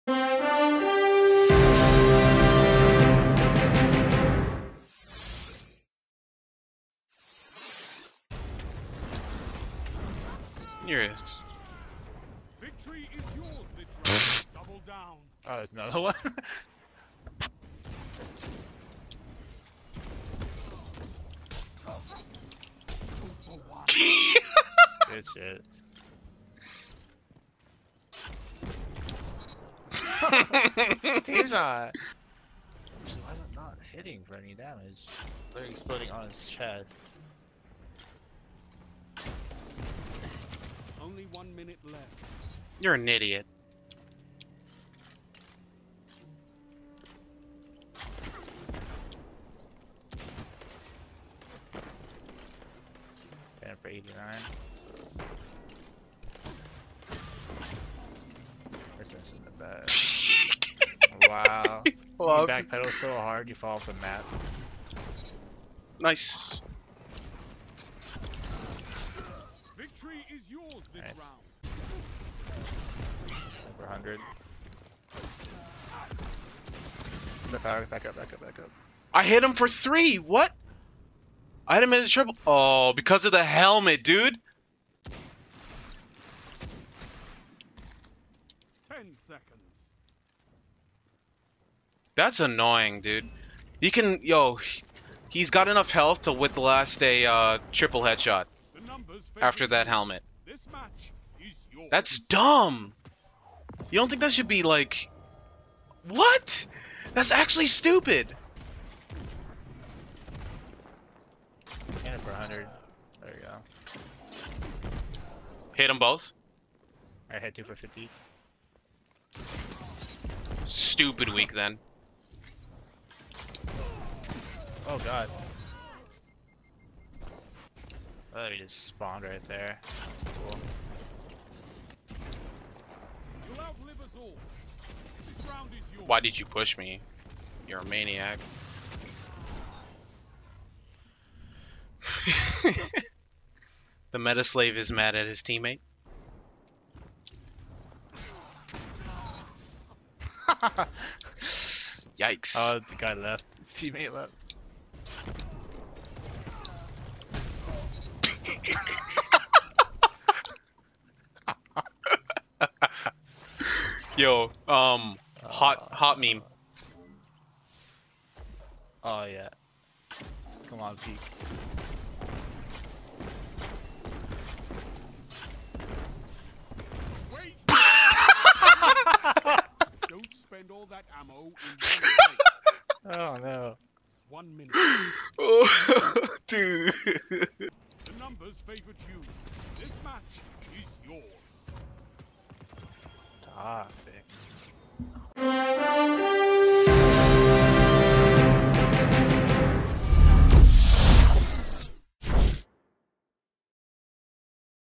Sum 2v2
Playing doubles with JustaaCasual. Out here with double lions.